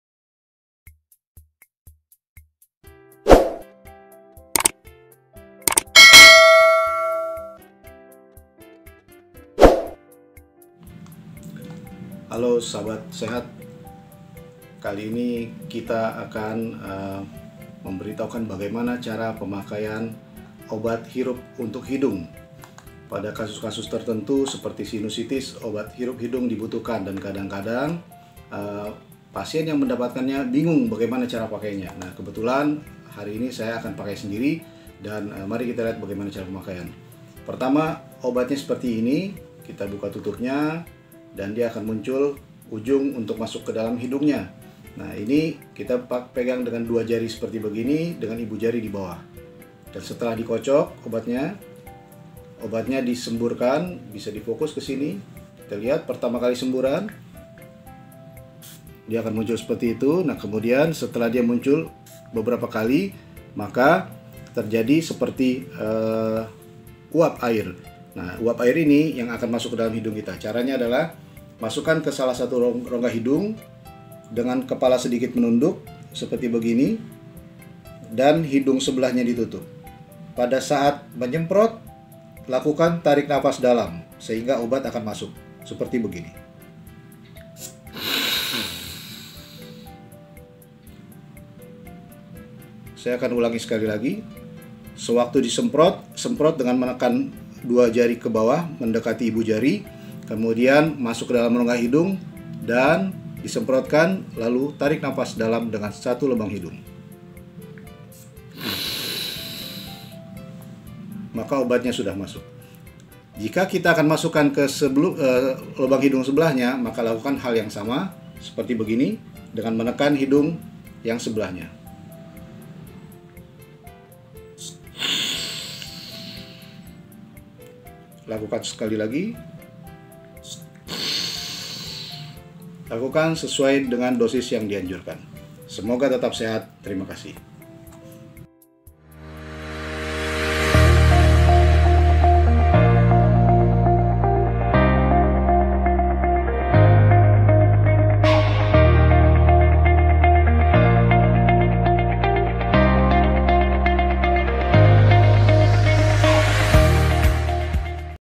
Cara penggunaan obat hirup/ semprot hidung
Video edukasi ini dibuat sebagai informasi tentang cara penggunaan obat semprot hidung. Ceritanya sedang jadi pasien, jadi sekalian saja saya share pengalaman ya.